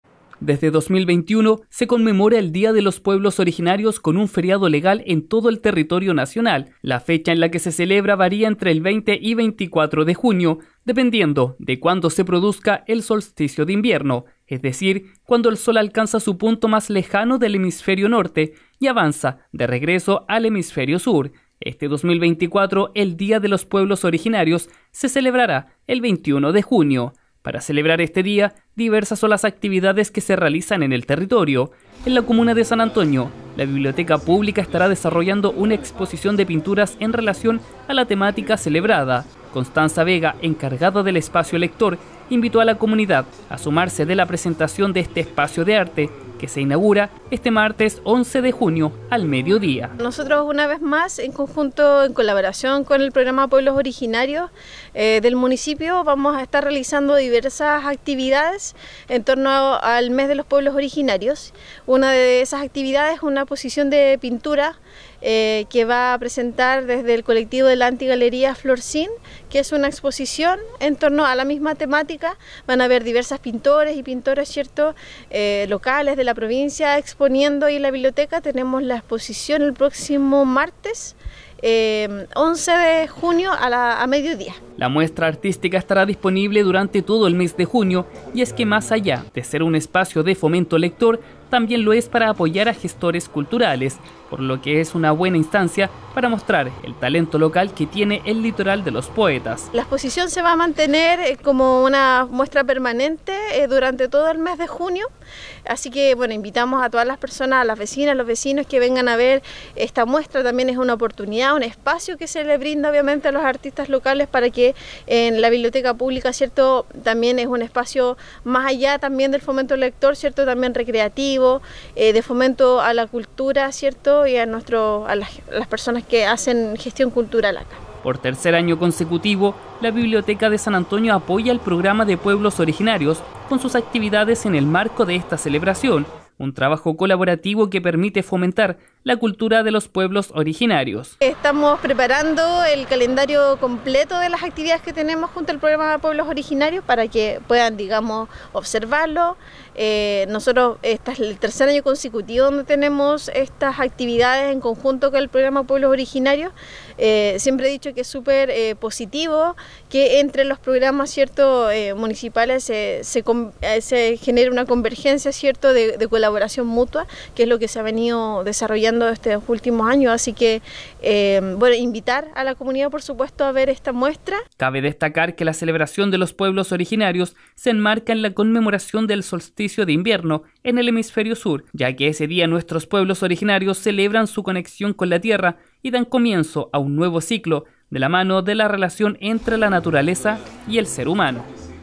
10 JUN 2024 Biblioteca de san Antonio se suma a la celebración del día de los pueblos originarios
La biblioteca pública de san Antonio se suma a la celebración del día de los pueblos originarios. Más allá de ser un espacio de fomento lector, también lo es para apoyar a gestores culturales, por lo que este próximo martes inauguraran una exposición artística enfocada en los pueblos originarios.